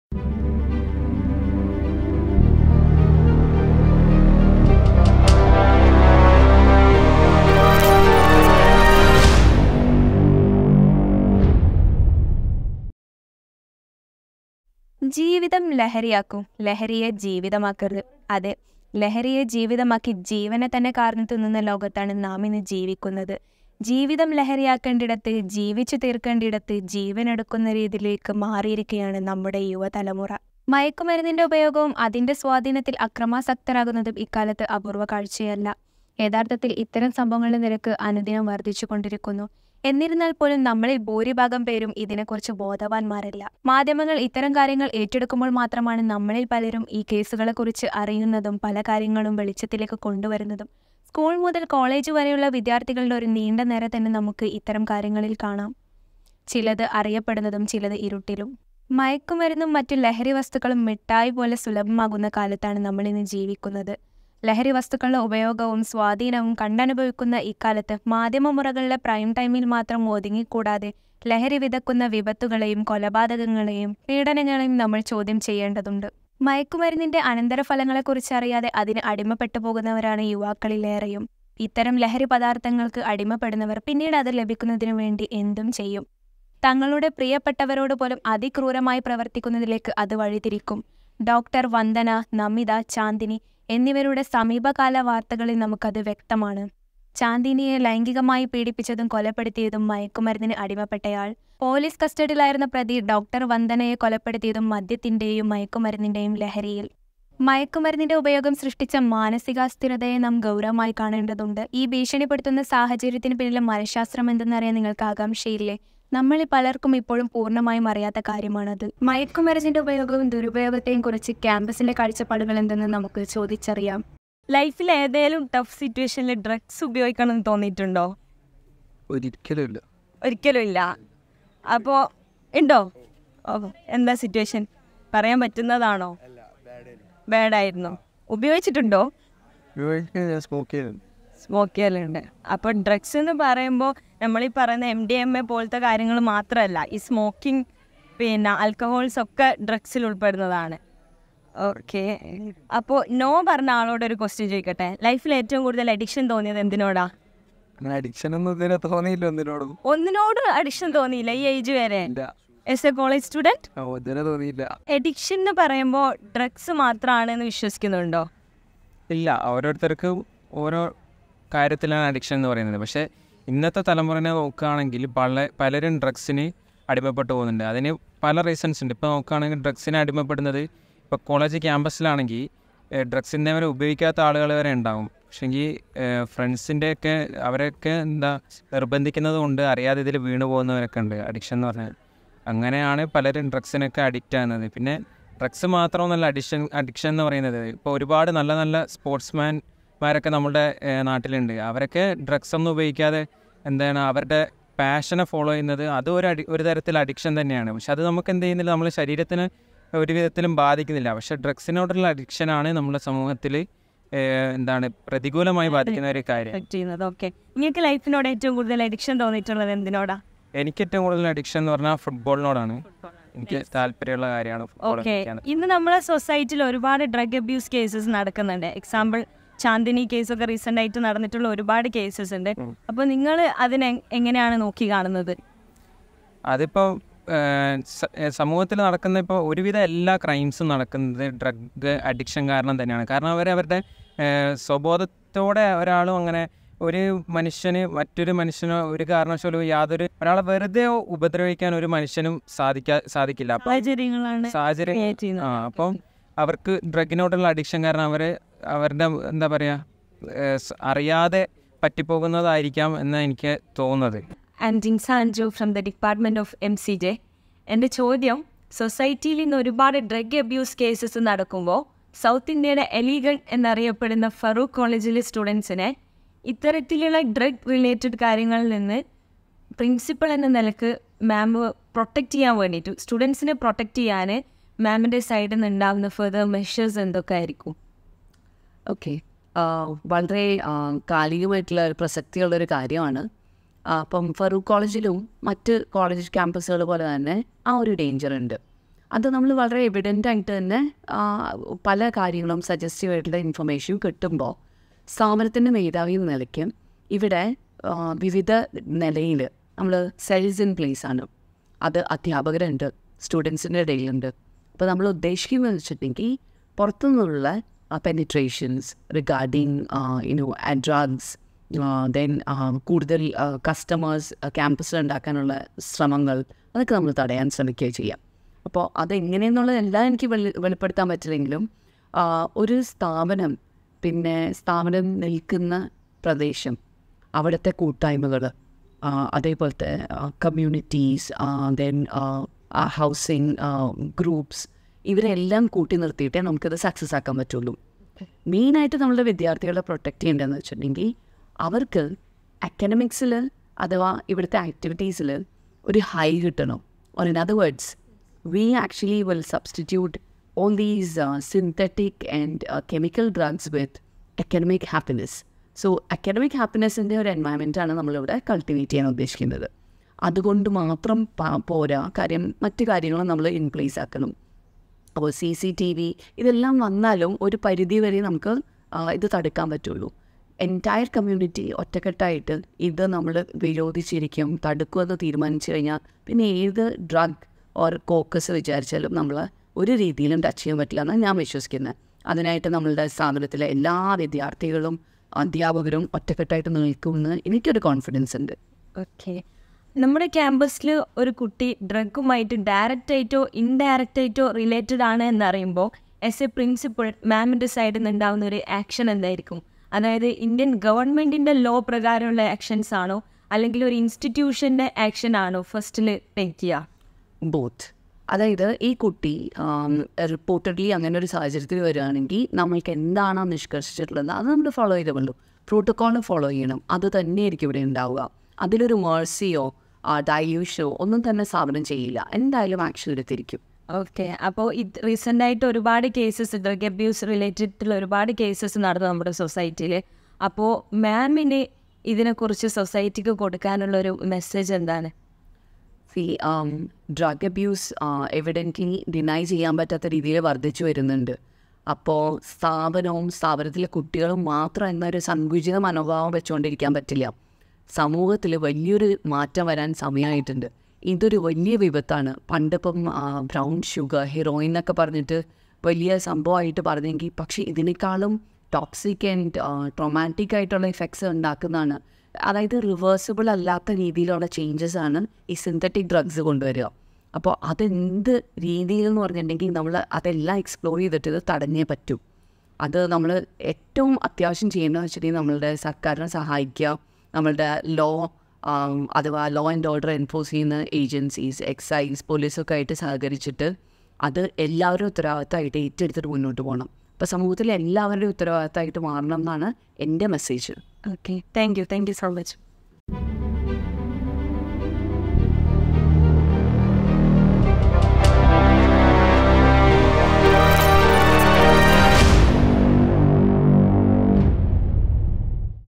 HOURGLASS | Episode 1 | Say No To Drugs | MCJ Dept | Farook College
PG Department of Journalism and Mass Communication presents a series of productions concerning the relevant issues happening in the society at present. In this episode, we are delving into the pressing issue of drug abuse within educational institutions through a series of candid interviews. Join us as we share the view points of our students and teachers, shedding light on their experiences, observations, and concerns regarding this issue.

Drugs are lethal weapons that can potentially eliminate all goodies of life. The business lobby has eyed humongous profits that this industry can yield, college students can potentially be the bait and carriers of these evil.

This insightful video aims to raise awareness and initiate meaningful conversations about the impact of drug abuse on academic environments.